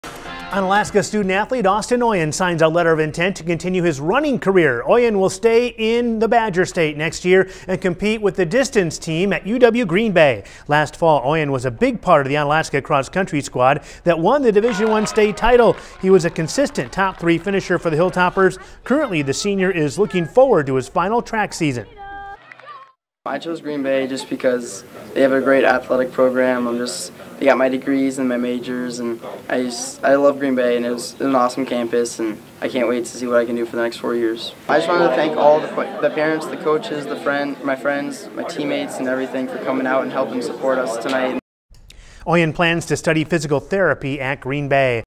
Onalaska runner Austin Oyen to continue running career at U-W Green Bay
Onalaska senior Austin Oyen has signed a letter of intent to compete with the distance team at U-W Green Bay on Tuesday afternoon.